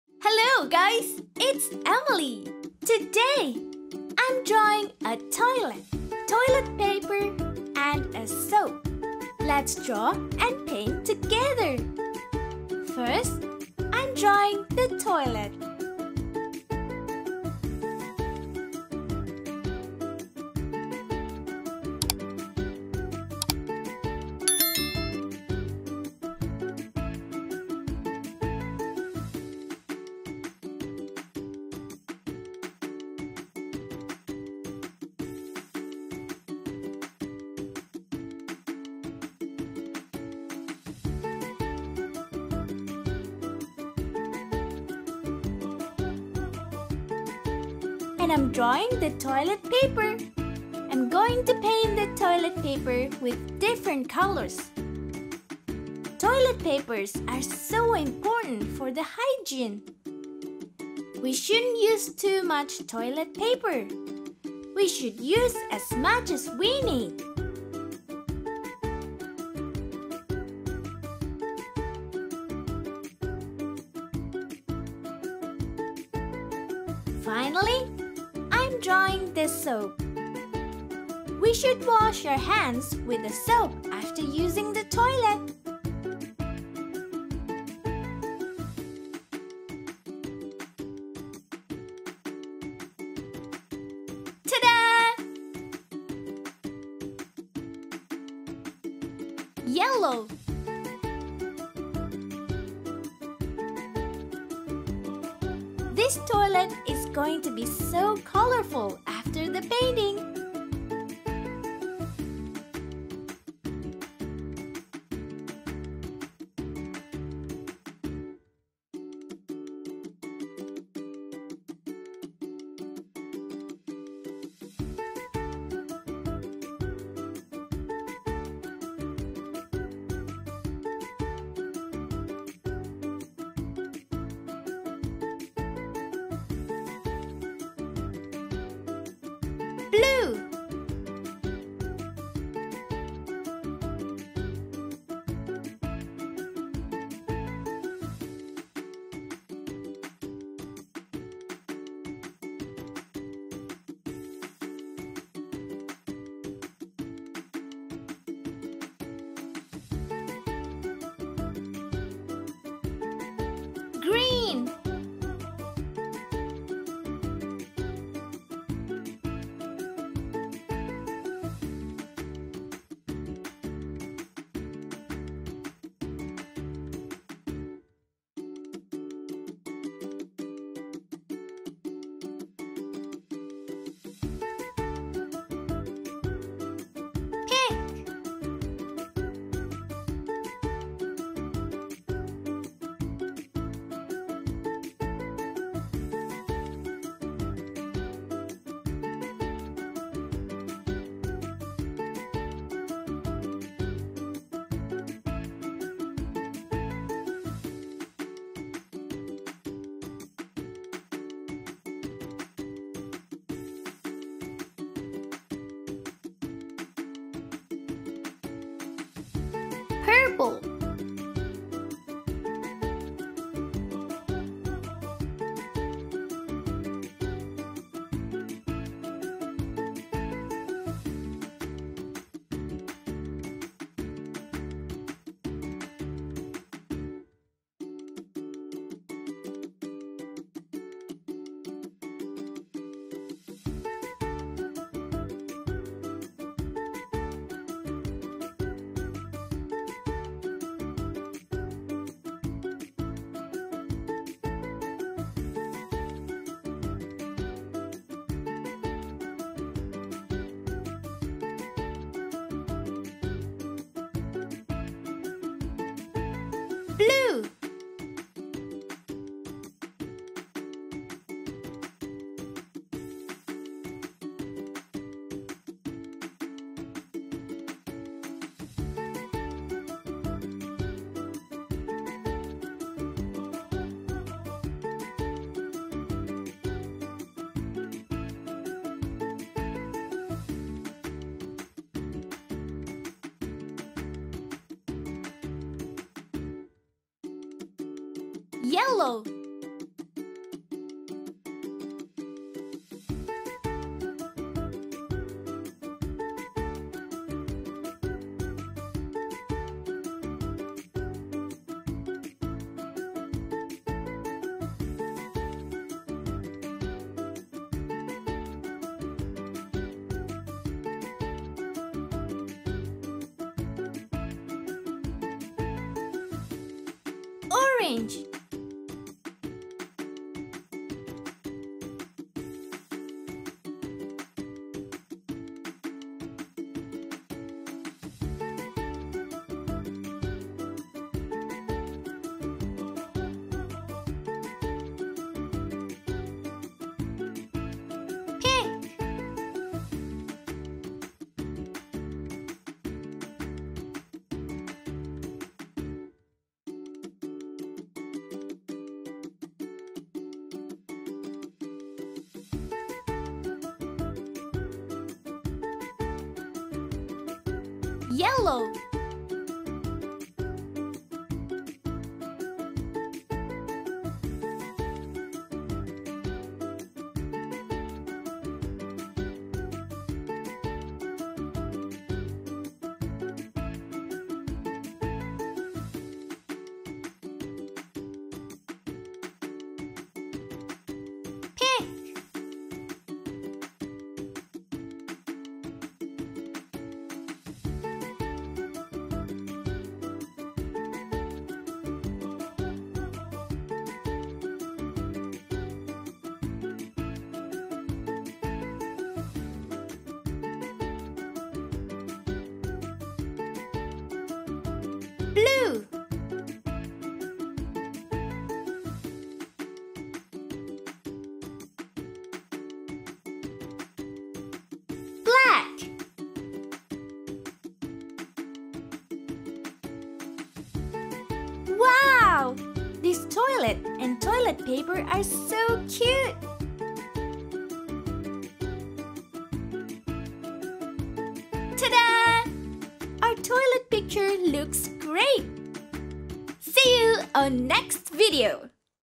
Toilet Drawing, Painting and Coloring for Kids & Toddlers | Drawing Basics #010
In our 'Toilet Drawing, Painting, and Coloring' tutorial, we delve into Drawing Basics for Kids & Toddlers. Witness as your little ones transform simple lines into a delightful toilet scene, promoting artistic exploration and imaginative play.

Foster a love for drawing with this interactive session designed for young artists.